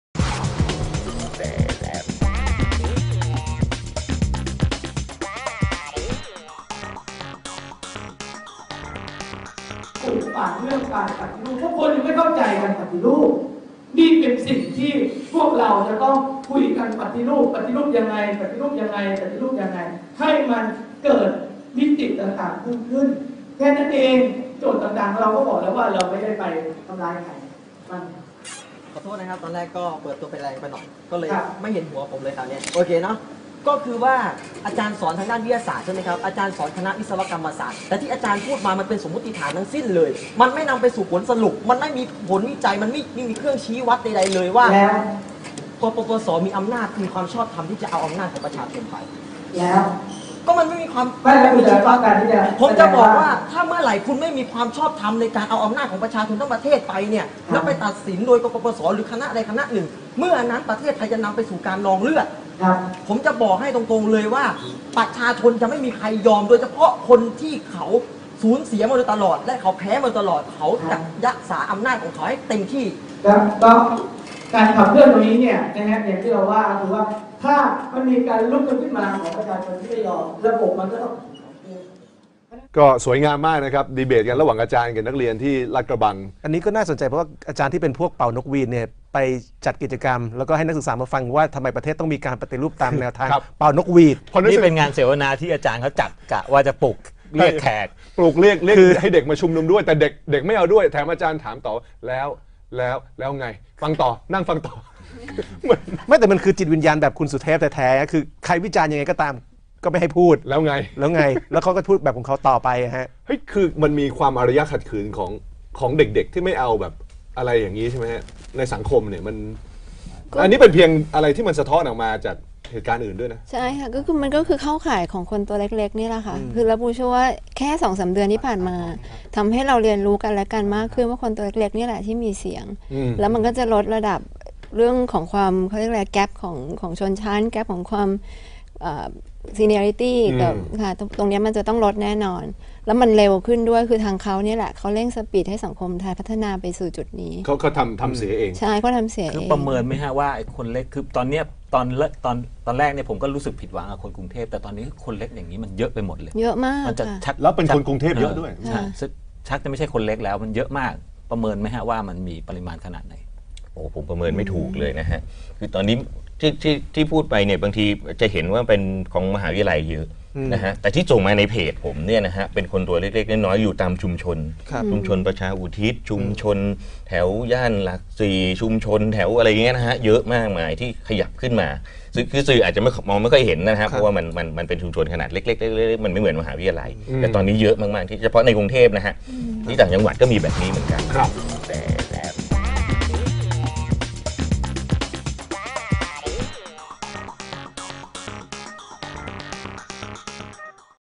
นักศึกษาลาดกระบัง ปะทะ อาจารย์ ข้องใจแนวทางของ กปปส.
ผู้ใช้ชื่อว่า  EDUREVIEW2013 ได้โพสต์คลิปในเว็บยูทูบ ซึ่งเหตุการณ์เกิดขึ้นที่คณะวิศวะ มหาวิทยาลัยเทคโนโลยีพระจอมเกล้าเจ้าคุณทหารลาดกระบัง มีการจัดเสวนาหัวข้อ "ลูกพระจอม กับ การมีส่วนร่วมในการปฏิรูปประเทศไทย" ที่จัดขึ้นโดย ผศ.ดร.คมสัน มาลีสี รองคณบดีคณะวิศวะ

10 มกราคม 2557